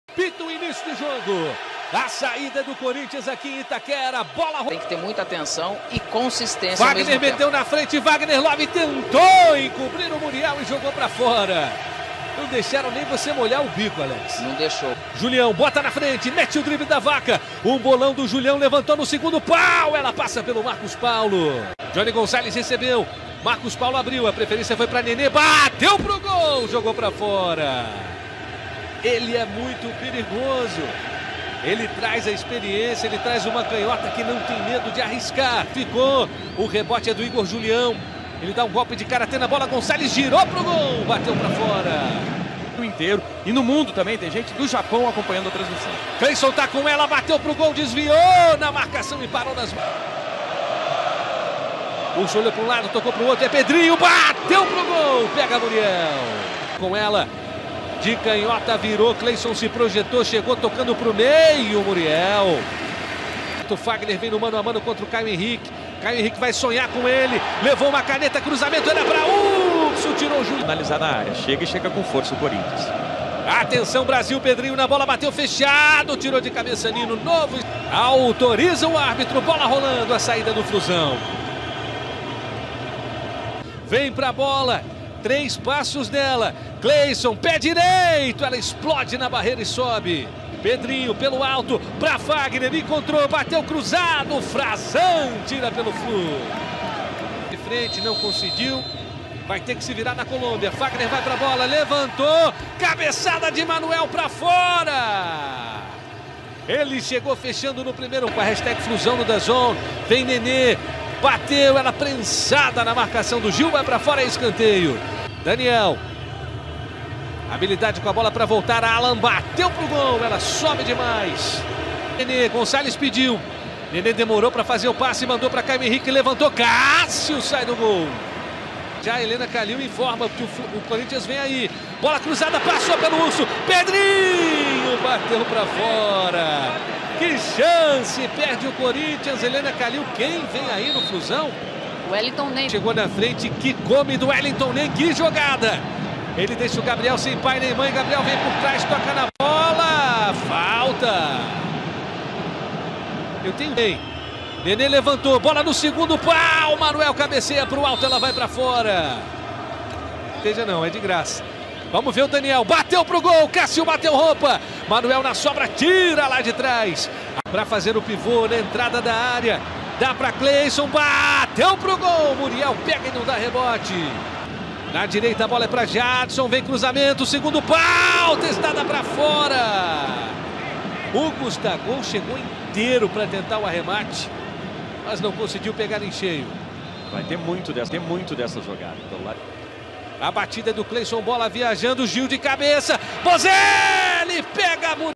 CORINTHIANS 0x0 flumin imagens dazn
FICHA TÉCNICA
CORINTHIANS 0 X 0 flum rj
copa sul americana 2019 quartas de final ida
Local: Arena Corinthians
Data: 21 de agosto, às 21h30
Árbitro: Andres Rojas, John Alexander Leon e Wilmar Navarro (COL)
Público/Renda: 37.550 total ( 37.266 pagantes 294 nao pag)/R$ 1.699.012,00
Gramado: Bom.
Cartão amarelo: Mateus Vital, Gabriel e Vagner Love (COR), Nenê e Caio Henrique (f)
Cartão vermelho: -

GOLS: -

CORINTHIANS: 12-Cássio; 23-Fagner, 26-Gil, 4-Manoel e 29-Danilo Avelar; 5-Gabriel e 11-Júnior Urso; 22-Mateus Vital (10-Jadson, 15’/2ºT),  18-Pedrinho (19-Gustavo 29'/2ºT), 25-Clayson; 9-Vagner Love (17-Boselli, 22’/2ºT). Técnico: Fábio Carille. + 27-Walter, 13-João Victor,  2-Michel Macedo, 6-Carlos Augusto, 15-Ralf, 24-Matheus Jesus, 28-Ramiro, 7-Sornoza, 20- Régis,

flum: 27Muriel, 21Igor Julião, 22Nino, 7Frazan e 19Caio Henrique; 6Allan, 20Daniel (13Caio, 33’/2ºT) e 17Ganso (23João Pedro, 41'/2ºT); 24Marcos Paulo (18Wellington Nem, 15’/2ºT), 4Nenê e 11Yony. Técnico: Marcão. 1Marcos Felipe, 25Agenor 26Digao 2Gilberto 15Dodi 10 Pablo Diego 16Mascarenhas 29Ze Ricardo